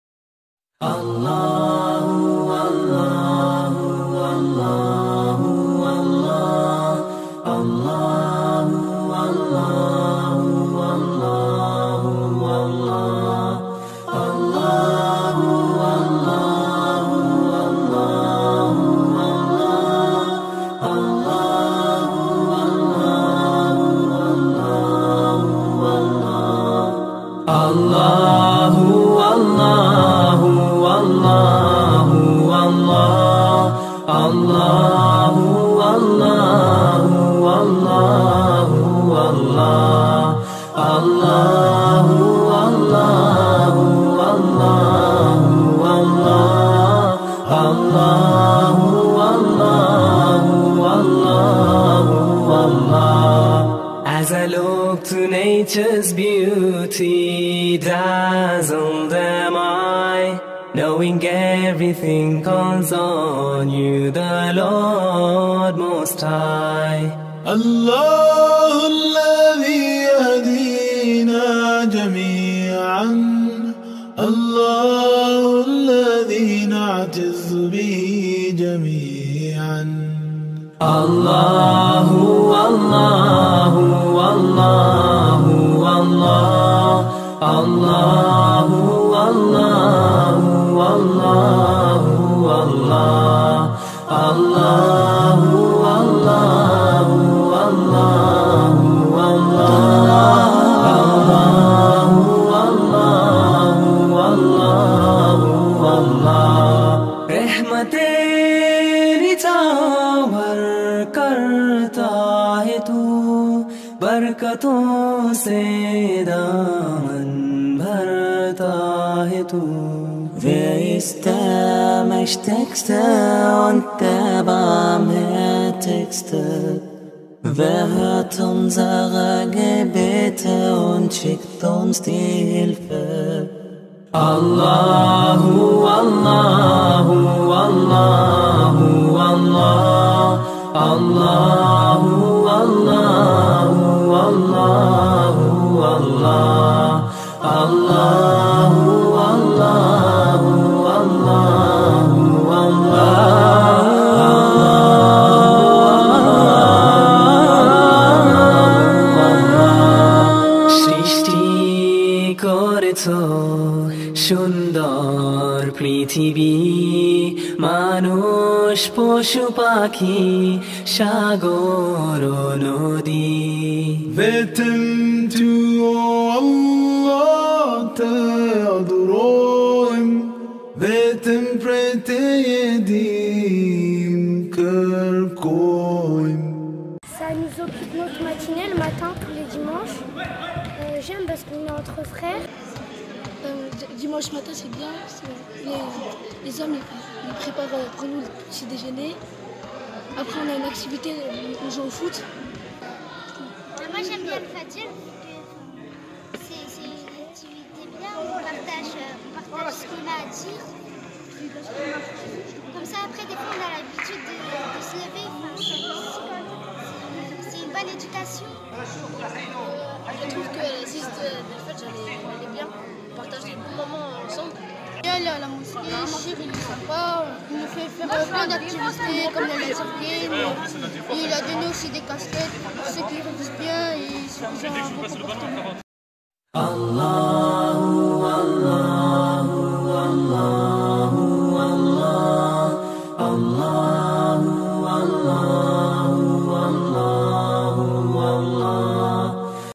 ACMSLT : Les matinées spirituelles du dimanche (CHOUROUK)
L'ACMSLT organise des matinées spirituelles tous les dimanches matin après la prière du Sobh.
Il s'agit de mettre en pratique une SOUNNA de notre prophète que le salut et la paix soient sur lui.
Plusieurs activités sont proposées dans une ambiance familiale et conviviale.
Tout est expliqué dans la vidéo.
N'hésitez pas à nous rejoindre.